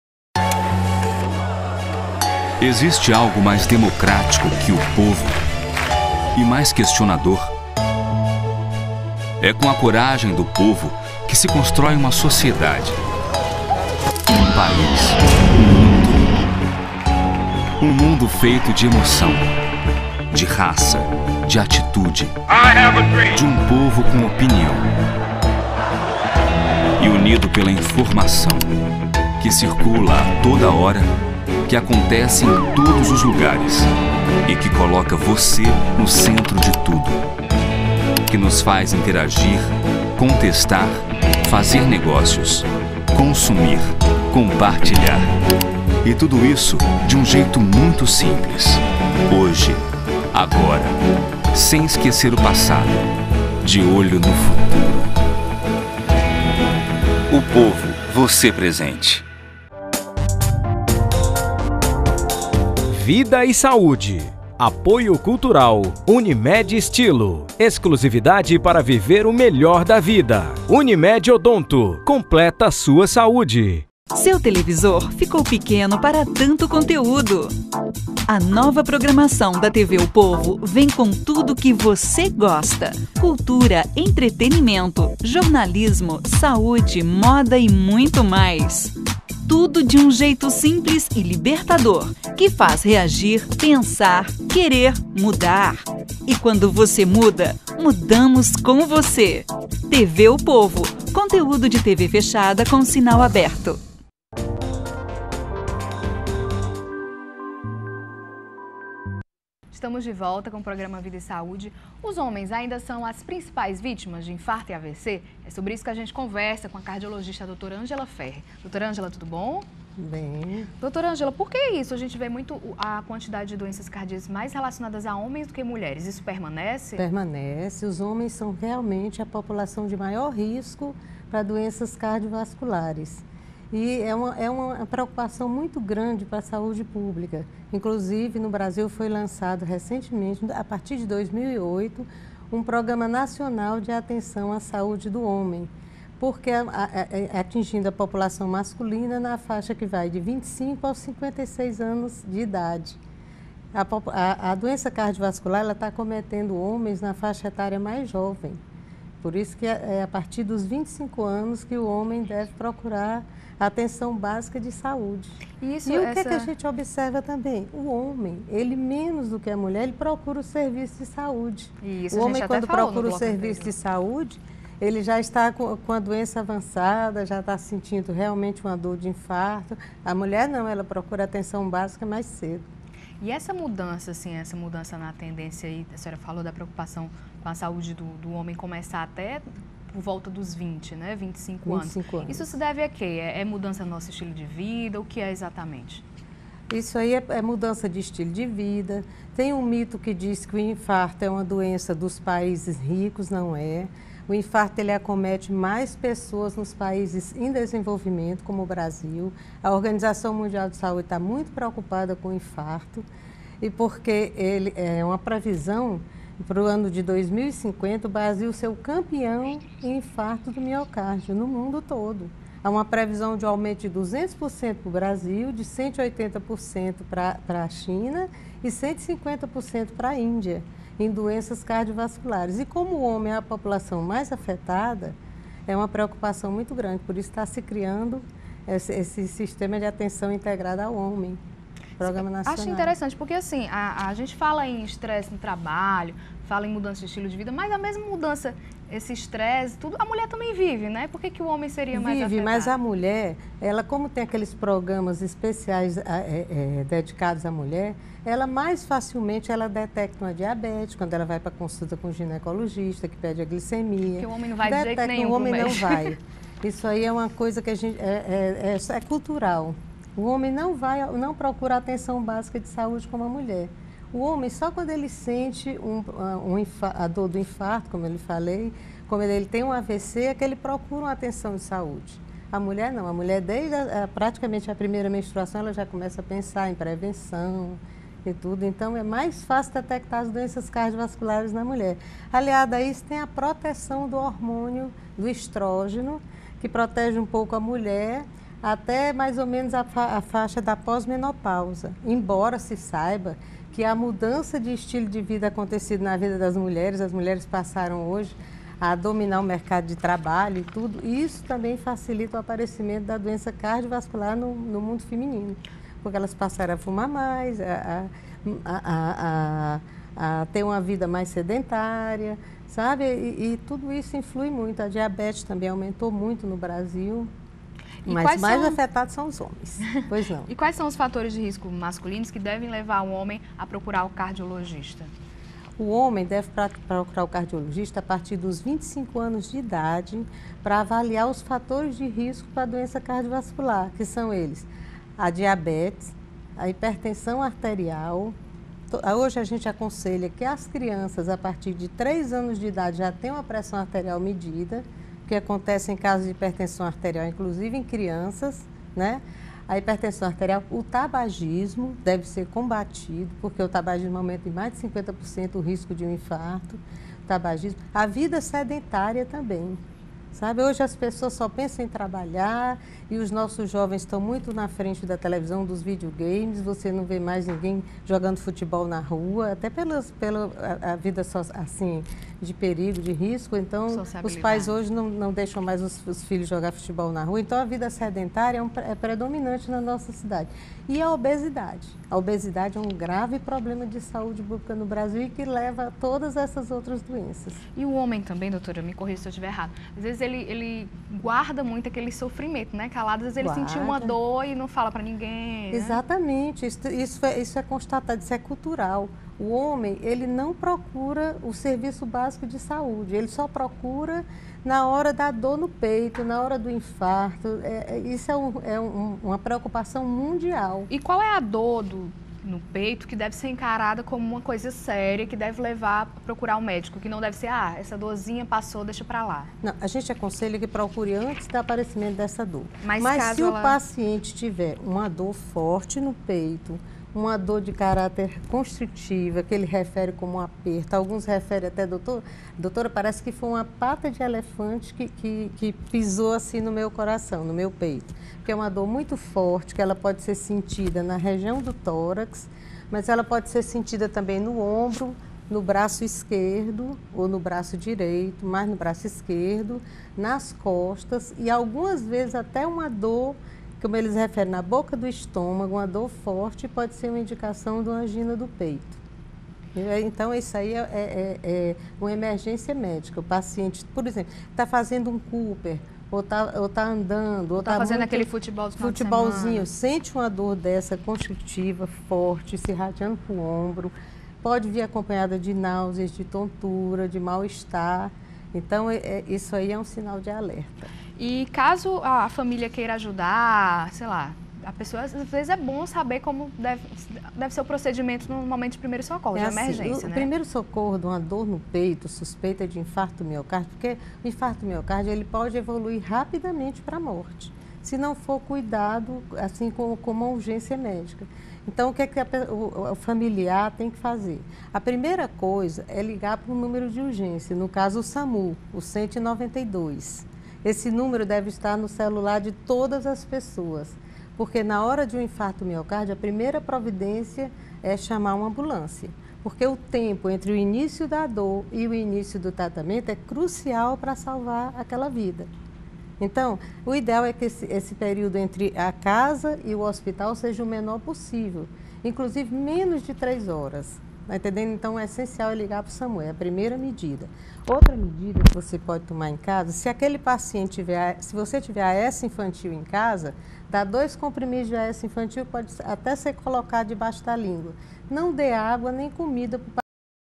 Saúde do homem - Vida e Saúde - 05.07.2012 - Parte 2
Diante de uma menor procura por médicos, a saúde masculina foi tema de discussão entre vários profissionais. O urologista, Dr. Ariel Scafuri, compara os hormônios masculinos com os femininos, as alterações que o corpo passa após tomar hormônios artificiais e a prevenção do exame de próstata. A cardiologista, Dra. Ângela Férrer, faz um alerta sobre doenças cardiovasculares, infarto, AVC e também estipula a idade certa para buscar um cardiologista. A alimentação necessária para o corpo masculino foi assunto debatido pela nutricionista Karla Pinheiro, que falou sobre a importância de alimentos no aumento de massa muscular e também no emagrecimento. O cirurgião plástico, Dr. Cido Carvalho, cita os procedimentos cirúrgicos mais procurados pelos homens e também comenta sobre os preconceitos que envolvem esse assunto. Confira estas e outras entrevistas.